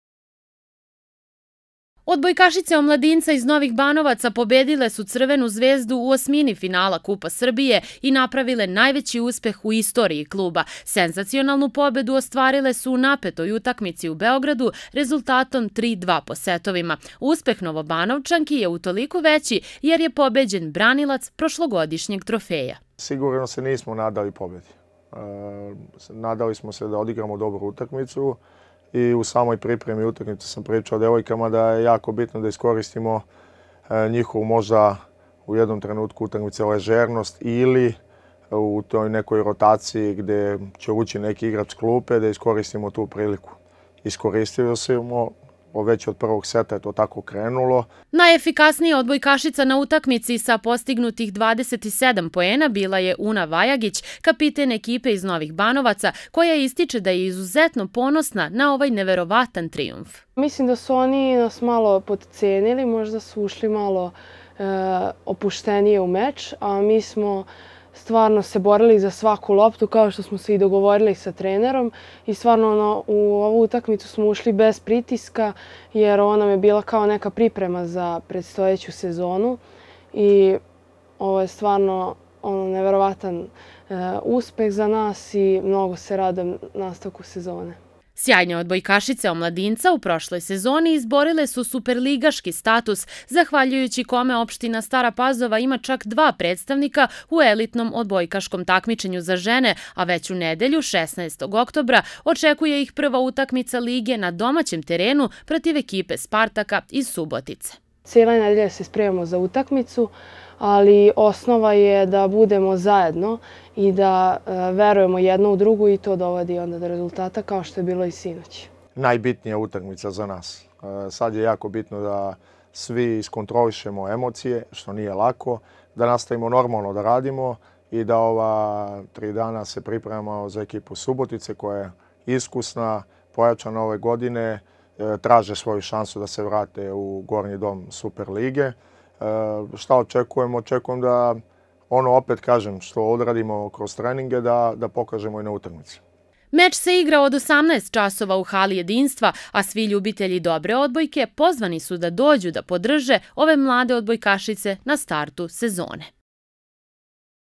Odbojkašice Omladinca pobedile Crvenu zvezdu
Odbojkašice Omladinca iz Novih Banovaca sinoć su postigle sigurno najveći uspeh od početka postojanja kluba, savladavši favorita Crvenu Zvezdu u Beogradu u osmini finala Kupa Srbije. U nedelju ovaj mladi tim očekuje takođe i prva utakmica prestižnog domaćeg takmičenja Superlige Srbije.